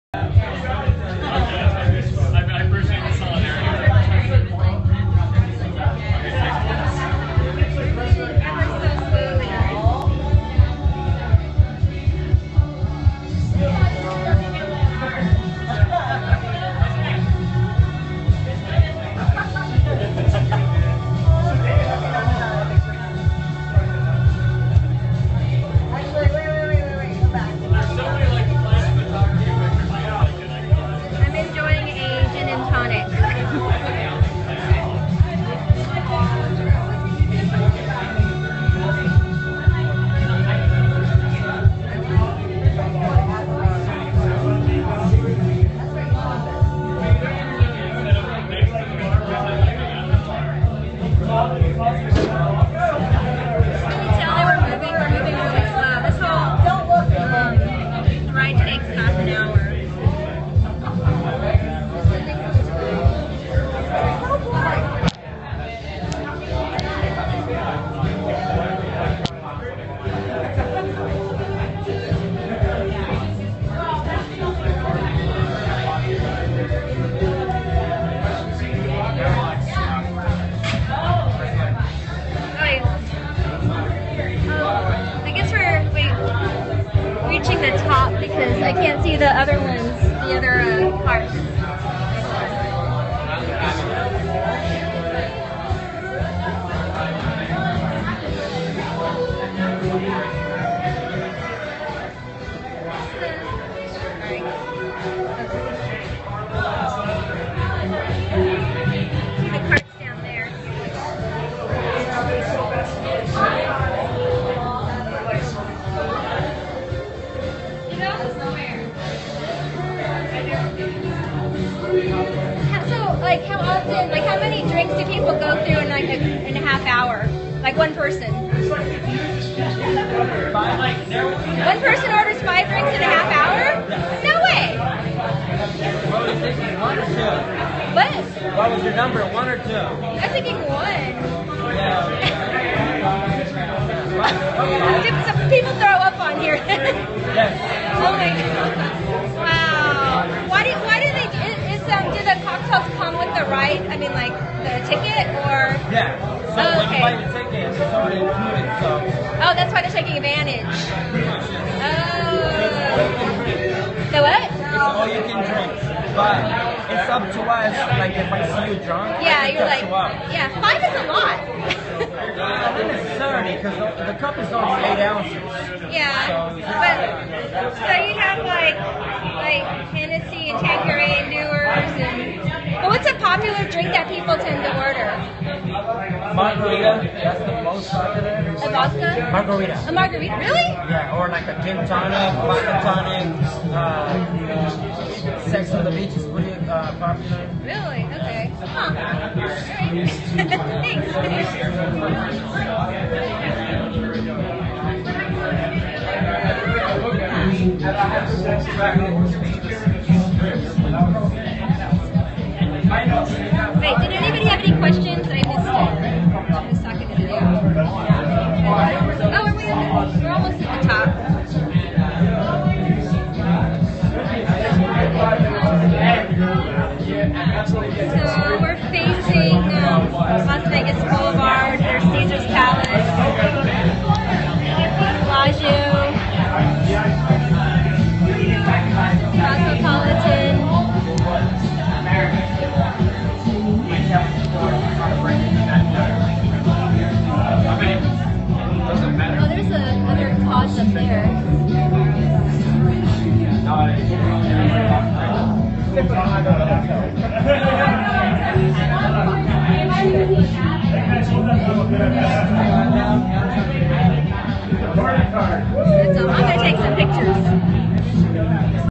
Hitting the bar in the High Roller in Vegas
Drinking several hundred feet over Vegas